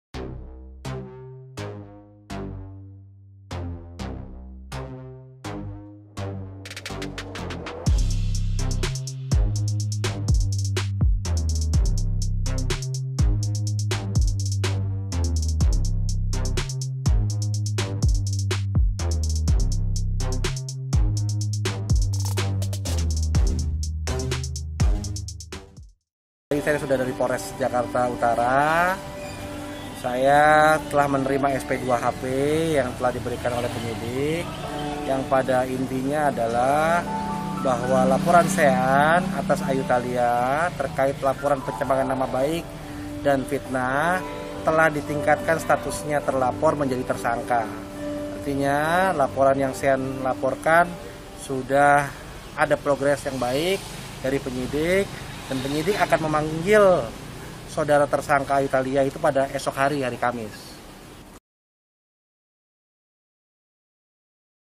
Kata Kuasa Hukum Terkait Status Ayu Thalia Tersangka Pencemaran Nama Baik Ngaku Dianiaya Anak Ahok
JAKARTA, KOMPAS.TV - Kuasa Hukum Nicholas Sean Purnama Ahmad Ramzy menjelaskan setelah menaikkan status kasus ke penyidikan. Penyidik telah menetapkan Ayu Thalia sebagai tersangka pencemaran nama baik.

Dengan penetapan tersangka ini, ramzy menilai penyidik sangat profesional dalam memproses pelaporannya. Dirinya menyatakan rencananya penyidik akan menjadwalkan pemeriksaan Ayu Thalia sebagai tersangka pada Kamis (20/1) ini

Baca Juga Sosok Ayu Thalia, Tersangka Pencemaran Nama Baik yang Pernah Ngaku Dianiaya Anak Ahok di

Seperti diketahui, kasus ini bermula dari selebgram Ayu Thalia melaporkan putra Basuki Tjahaja Purnama atas tuduhan penganiayaan ke Polsek Penjaringa. Karena tidak ada bukti dan permintaan maaf, sean melaporkan balik Ayu Thalia dengan pasal pencemaran nama baik.

Video Editor: Androw